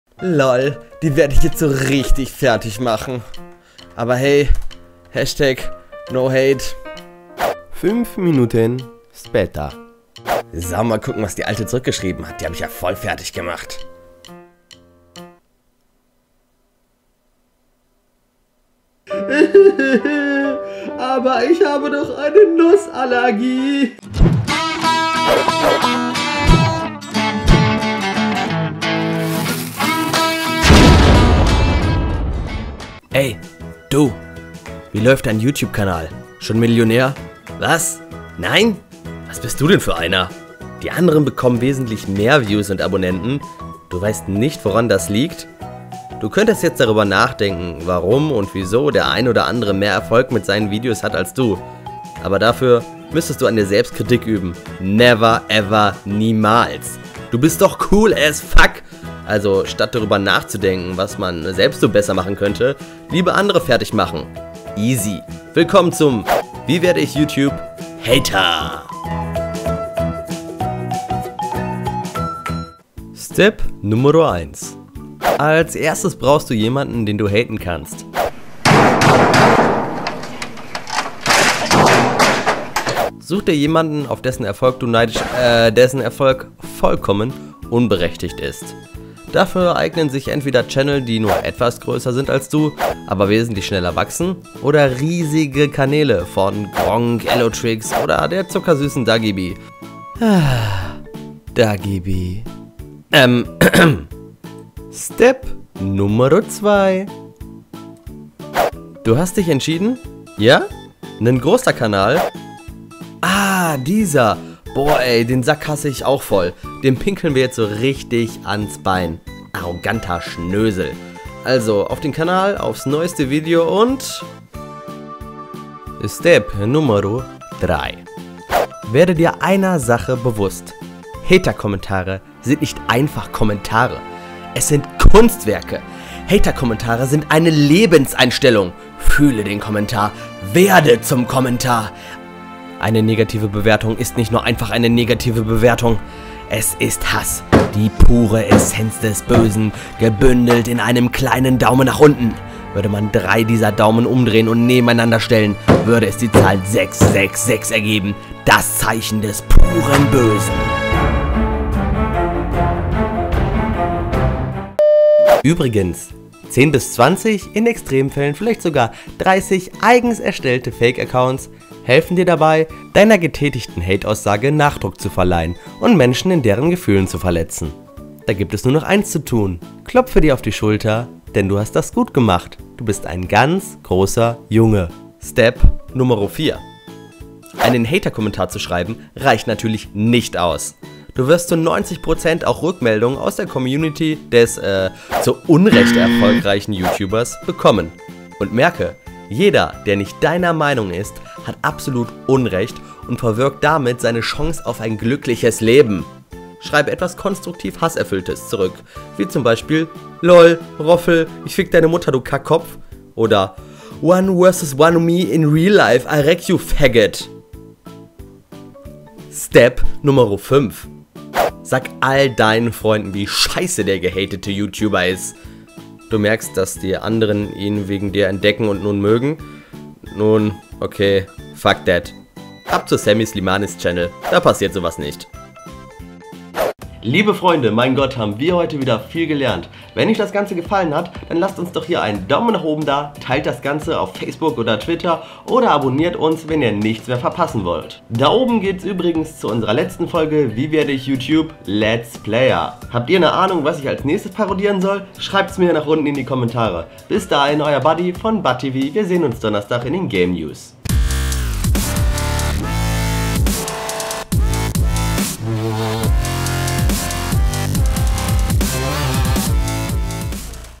Youtuber werden? Hater - Wie Hate ich Dagi Bee und Sami Slimani - Tutorial | BudTV Triple D
Wie werde ich Youtuber ? Hater - Wie Hate ich Dagi Bee und Sami Slimani - Tutorial | BudTV - Nachdem der erste Teil super angekommen ist, wir unglaublich viele positive aber auch einige negative Bewertungen bekommen habe, ist uns eines aufgefallen : Keine negativen Kommentare zu den Daumen nach unten ! Das geht natürlich nicht, deswegen zeigen wir euch heute: Wie HATE ich richtig.
Danke an die DaniLP übrigens für ihren tollen Konterkommentar - Made my Day!

Falls das ganze euch gefallen hat lasst doch mal ein Like & Abo da.
Eure Jungs von BUDTV